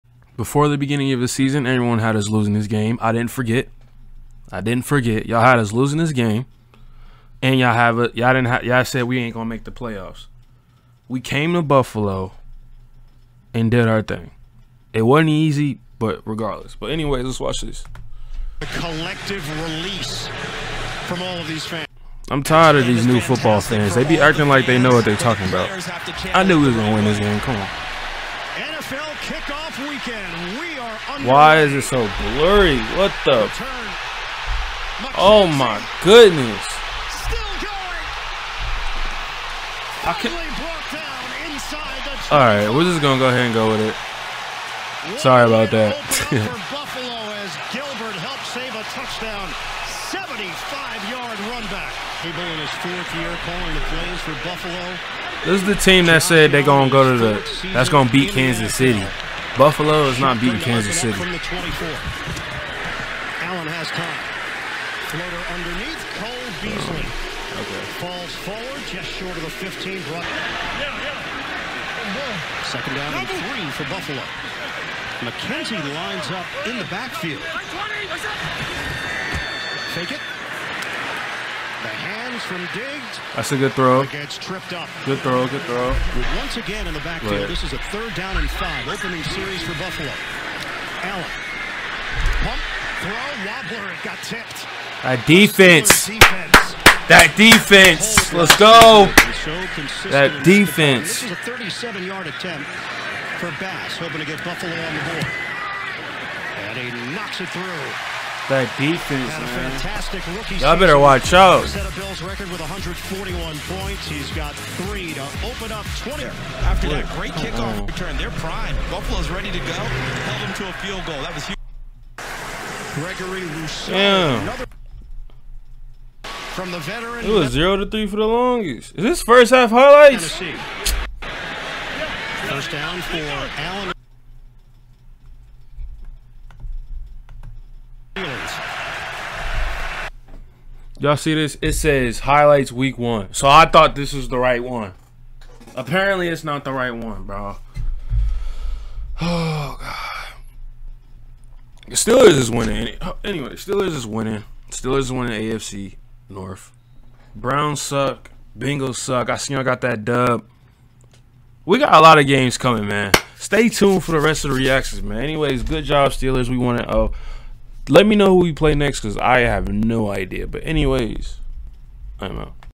STEELERS 1-0!!! 😈😁 Steelers vs Bills | Week 1 | 2021 NFL Highlights!
Pittsburgh Steelers vs Buffalo Bills Highlights Week 1 2021

Follow "Weefo" On IG, Twitter & TikTok!

by permitting the unlicensed use of copyright-protected works in certain
uses—such as criticism, comment, news reporting, teaching, scholarship, and